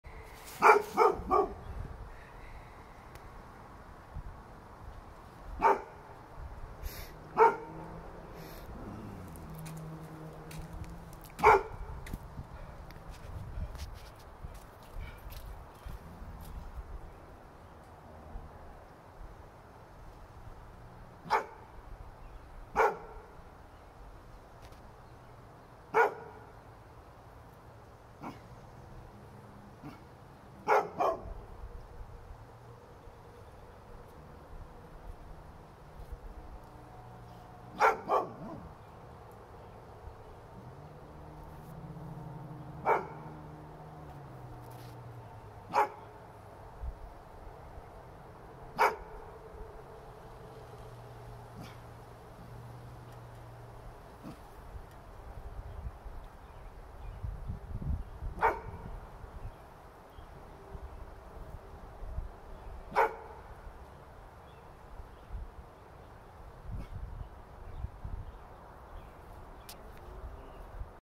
I Wish I Had Seen It
My Sister-in-law leaves her dog for us to babysit. She loves to walk and play with Bullet our dog.